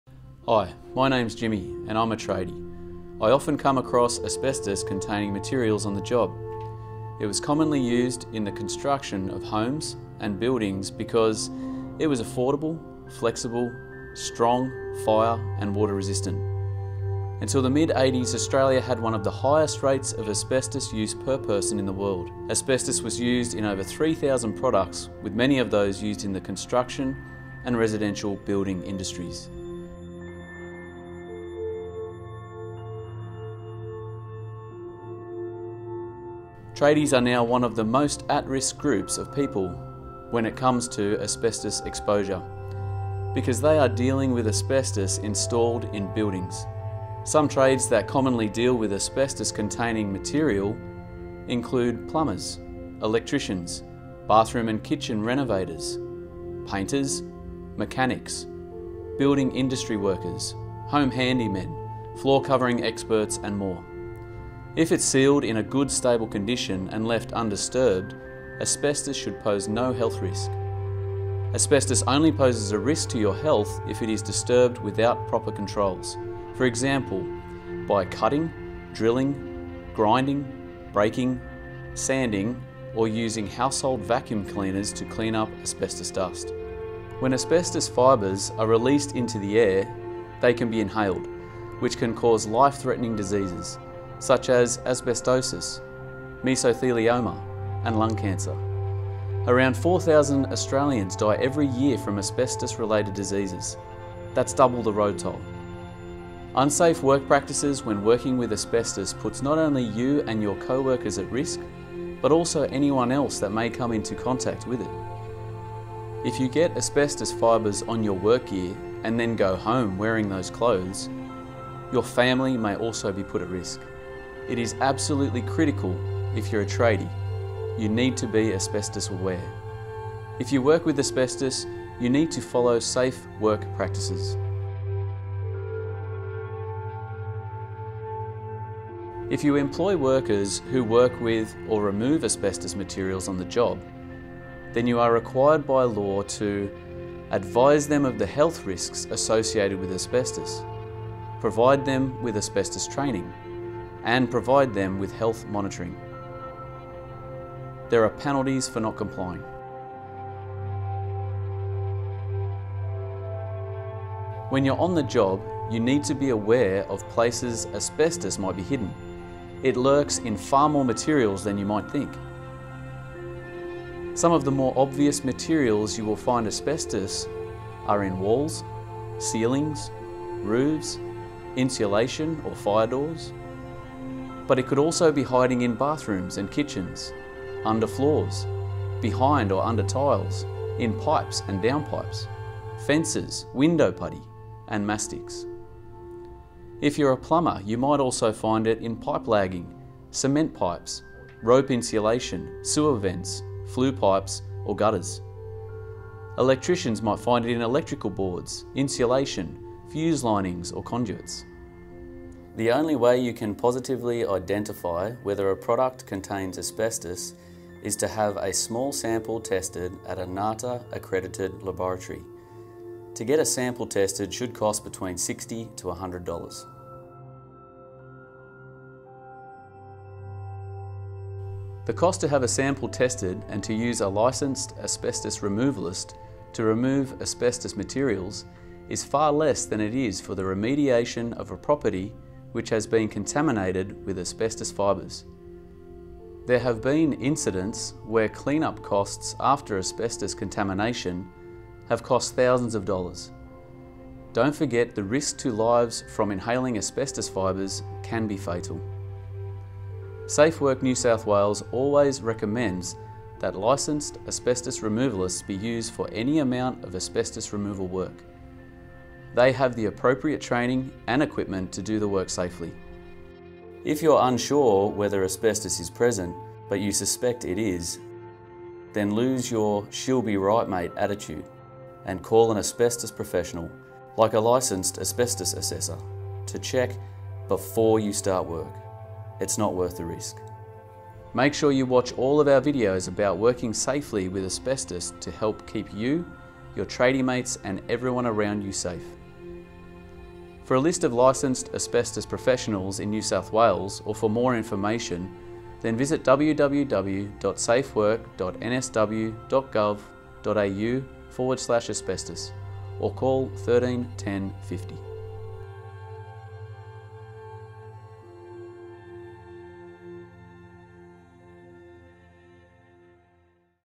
Video 1 – Asbestos Hazards, Risks and Identification
This is the first of five videos in a series, showing tradies how to manage asbestos safely on the job. This video covers the hazards and risks associated with asbestos and includes images of many asbestos containing materials.